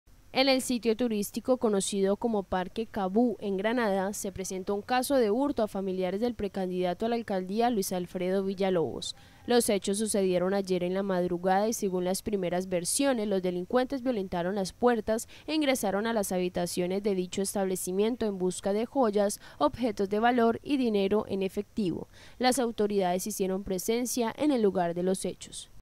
PRECANDIDATO A LA ALCALDÍA VICTIMA DE ROBO
En el sitio turístico conocido como  Parque Kabod en Granada, se presentó un caso de hurto a familiares del  precandidato a la Alcaldía,  Luis Alfredo Villalobos.